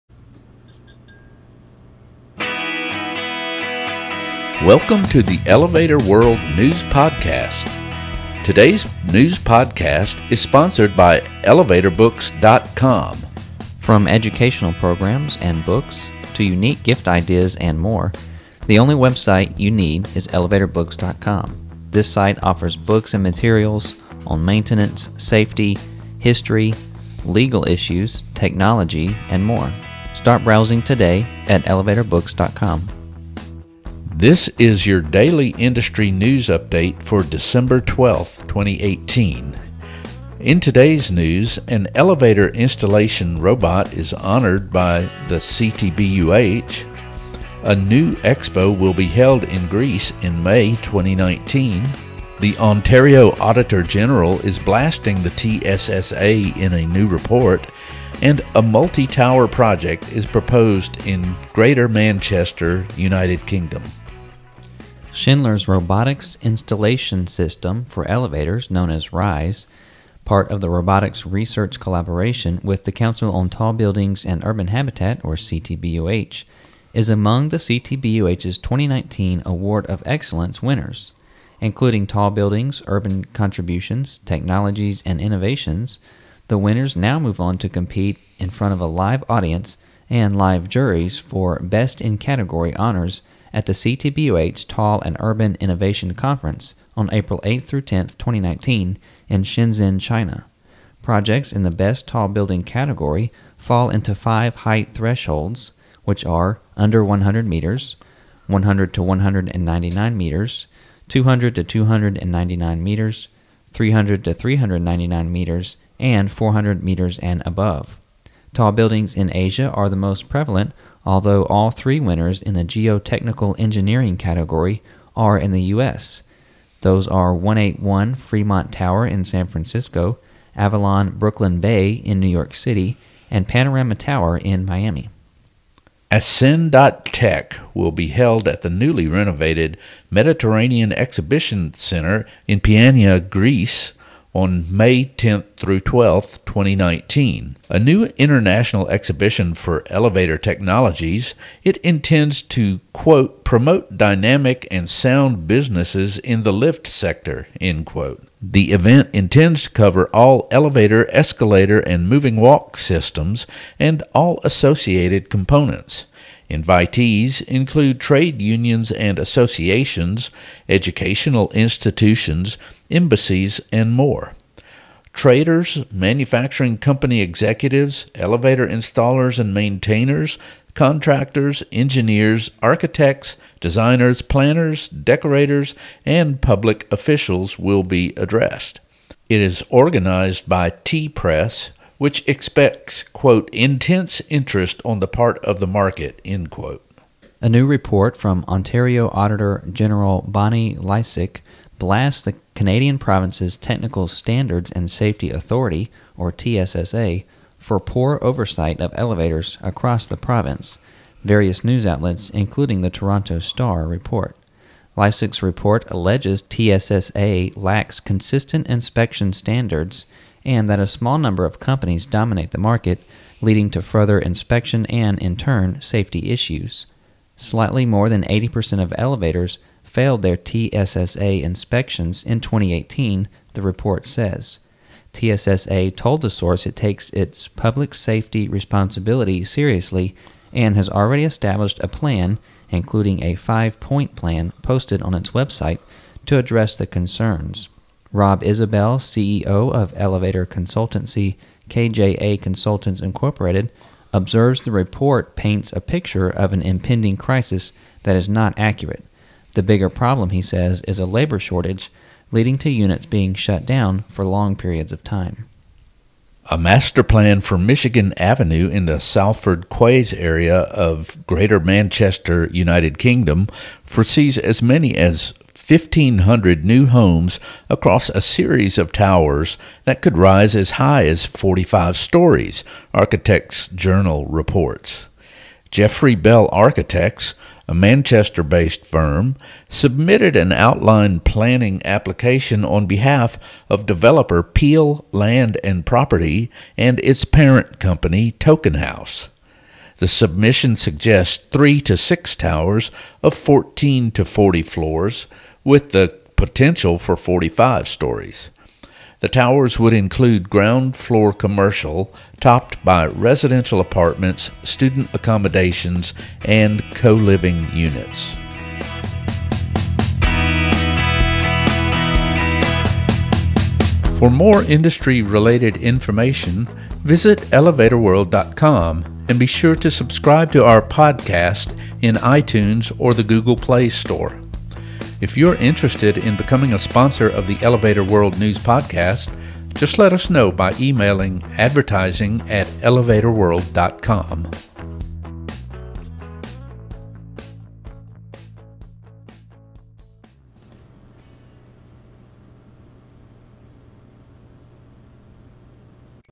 Elevator Installation Robot Among CTBUH Honorees
Welcome to the Elevator World News Podcast.

Schindler's Robotics Installation System for Elevators (R.I.S.E), part of a robotics research collaboration with the Council on Tall Buildings and Urban Habitat (CTBUH), is among CTBUH's 2019 Award of Excellence winners. Including tall buildings, urban contributions, technologies and innovations, the winners now move on to compete in front of a live audience and live juries for "Best in Category" honors at the CTBUH Tall + Urban Innovation Conference on April 8-10, 2019, in Shenzhen, China. Projects in the Best Tall Building category fall into five height thresholds (under 100 m, 100-199 m, 200-299 m, 300-399 m, and 400 m and above). Tall buildings in Asia are the most prevalent, although all three winners in the Geotechnical Engineering category are in the U.S.: 181 Fremont Tower in San Francisco, Avalon Brooklyn Bay in New York City and Panorama Tower in Miami.

Image: Schindler/CTBUH

To read the full transcript of today's podcast,